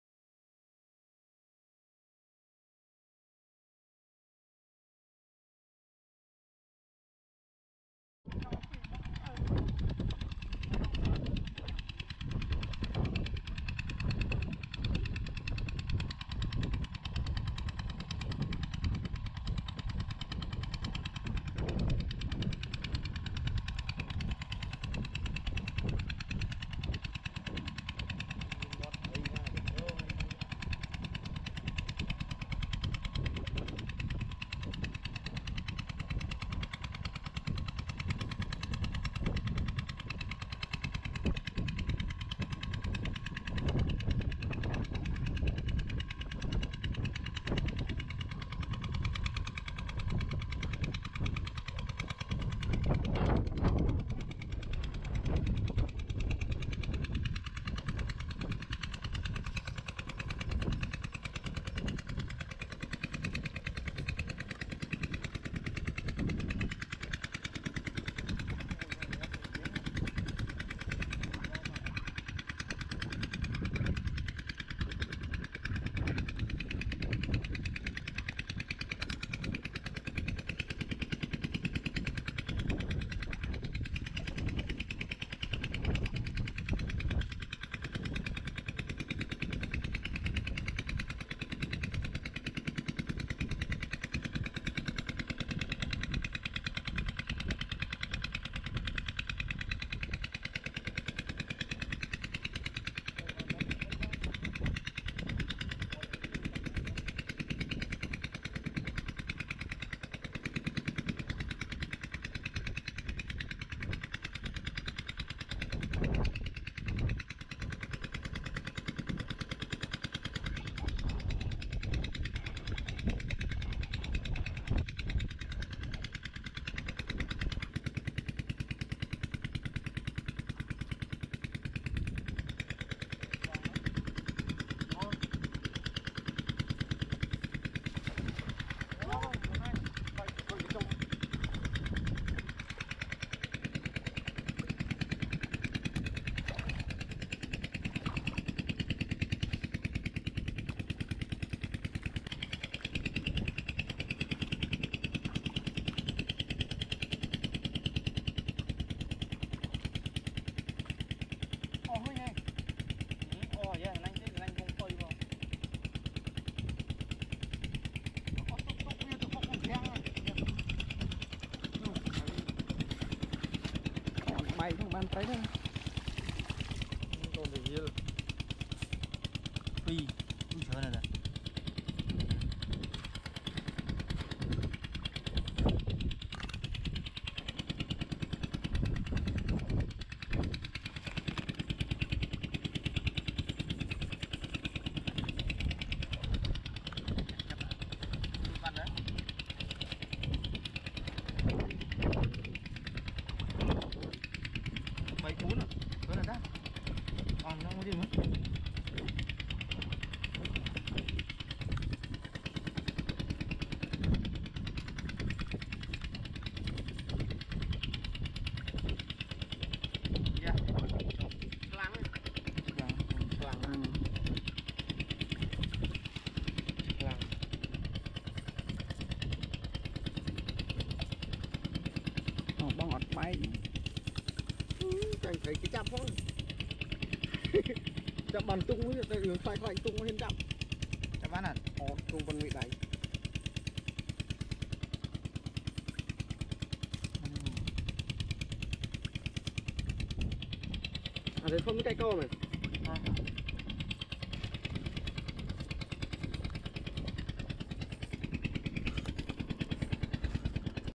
River Net Fishing Part 2 | Cambodia 2019
*** Fishing With Natural In Cambodia By Using NET  ***
River Net Fishing 2019
Net Fishing In Cambodia 2019 | NET Catching Fish

This video want to show how the way that Cambodian Fishing Using Net With The Real Natural And Fresh Fish From River.

This is the daily life of Fisher!